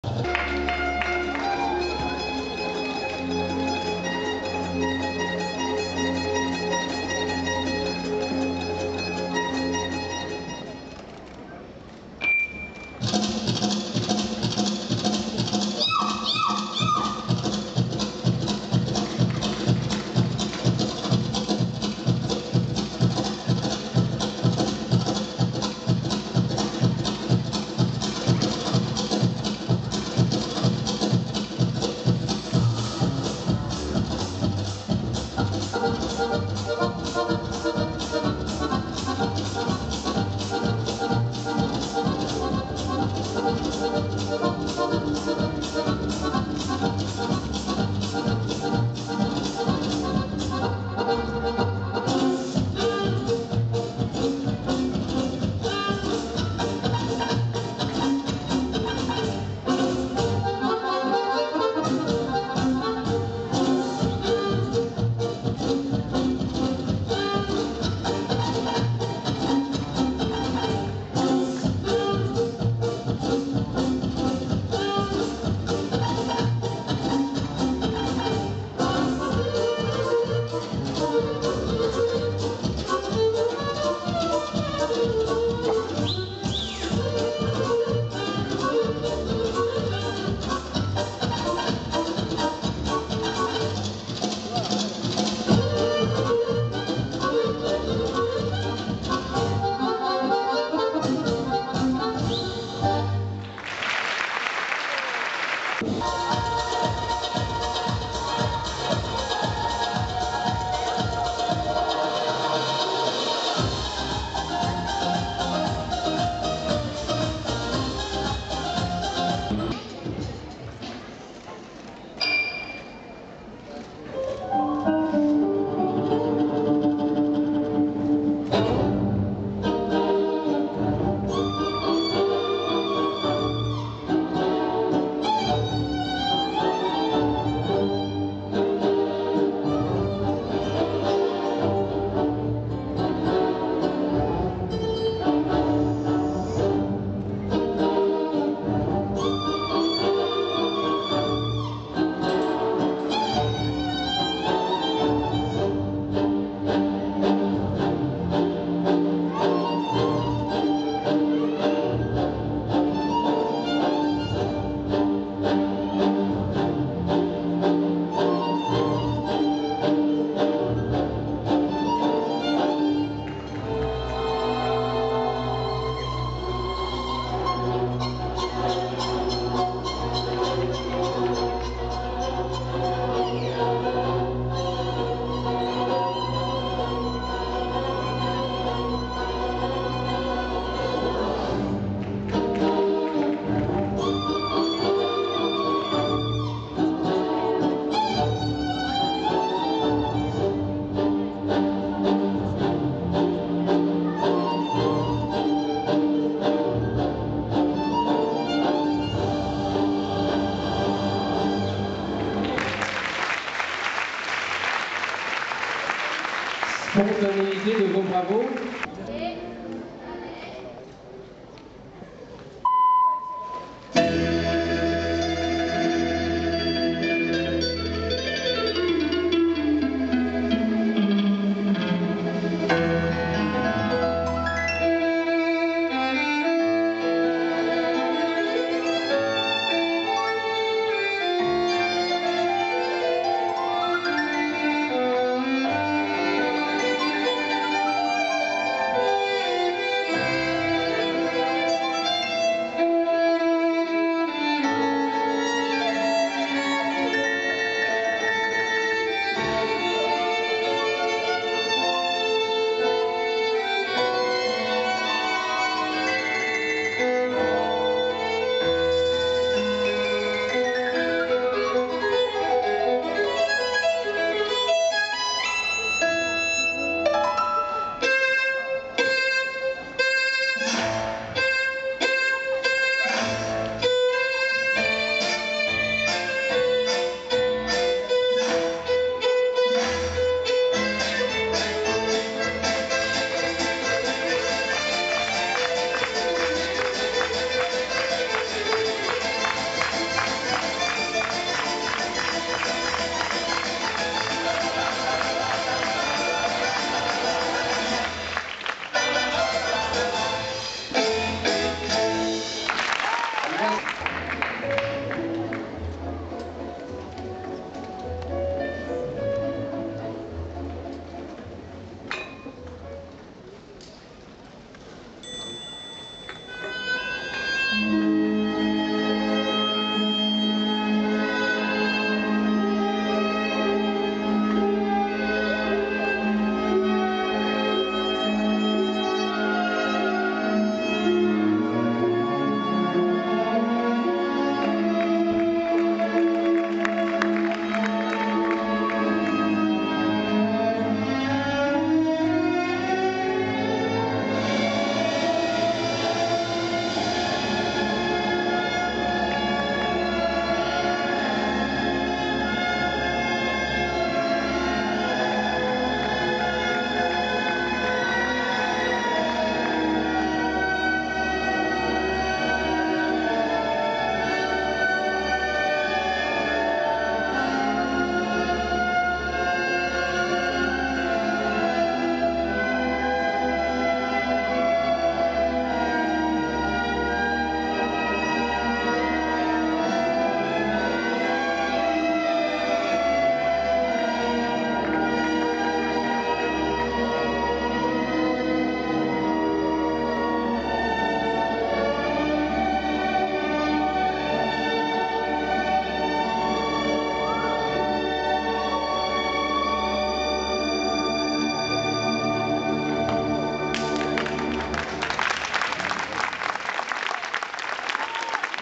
The outstanding Dara DMITRIEVA (RUS) 4 routines at Corbeil Essonnes, FRANCE 2009.
Internationaux de Corbeil Essonnes (FRANCE).
Concours général groupe B.
Dara DMITRIEVA (RUSSIE) à la corde, au cerceau, au ruban et au ballon.